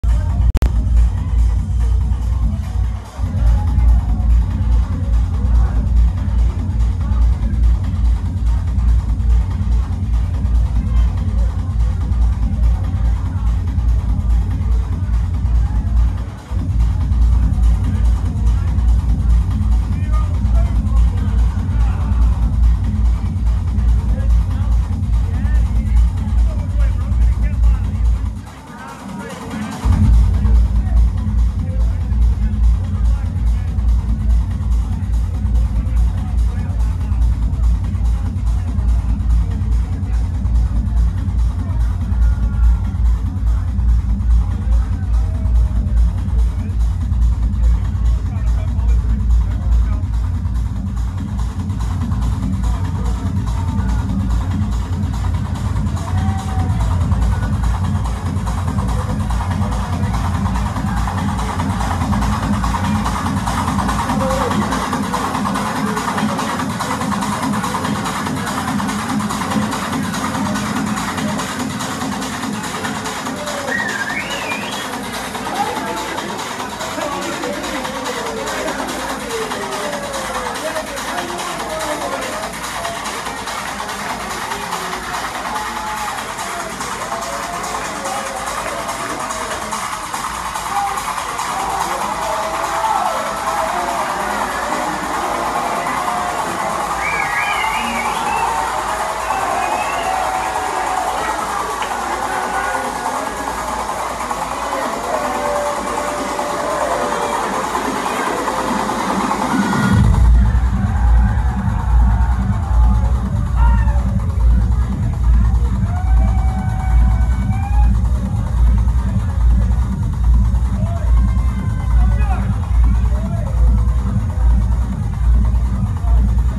Klangkuenstler @ Wonderpot x Kontakt @ 24 Kitchen St, Liverpool, UK, 17/09/2021 [4]
Klangkuenstler playing at Wonderpot x Kontakt @ 24 Kitchen St, Liverpool, UK, 17/09/2021